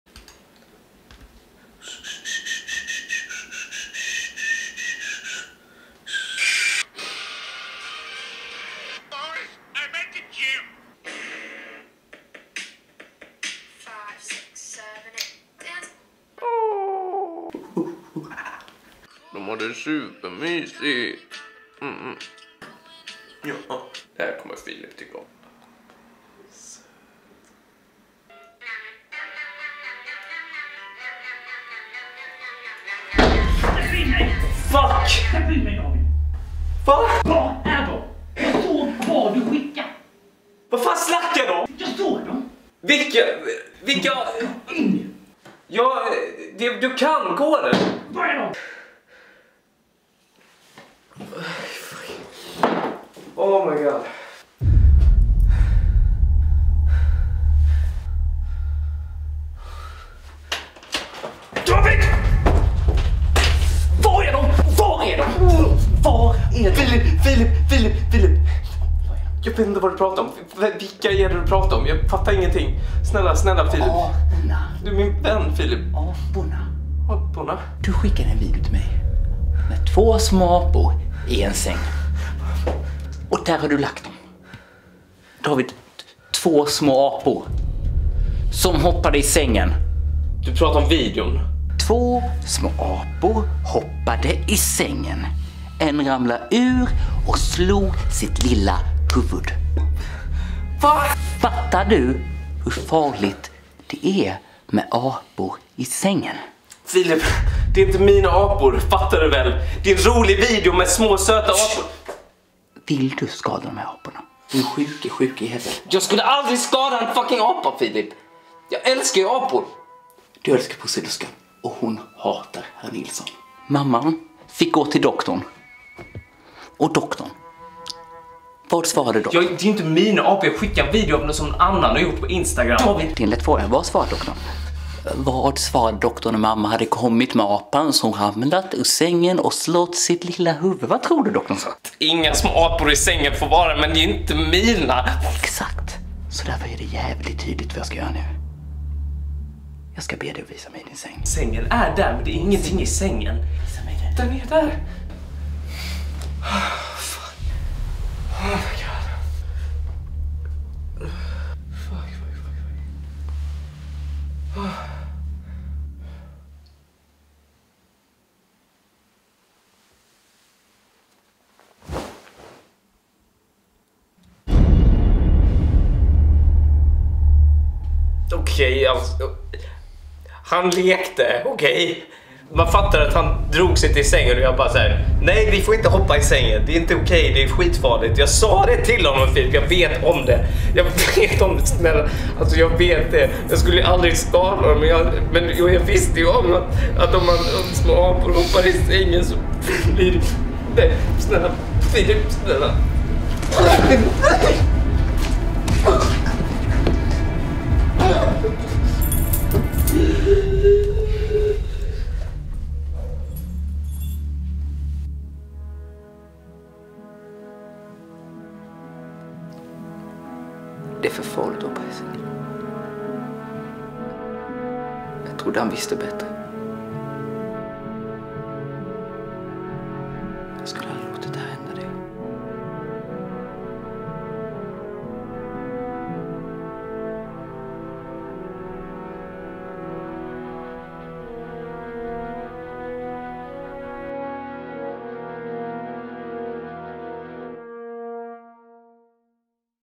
På Läkarens Order
Du vet känslan när man får ett kul klipp på insta?
Kanske ett gulligt klipp på två små apor?  Hur hade du reagerat?
Hade du blivit glad? kanske skrivit: hahahaha åhhh! ❤️
Eller... hade du blivit. J Ä V L I G T ... F Ö R B A N N A D ?

Medverkande

David Digman
Filip Johnsson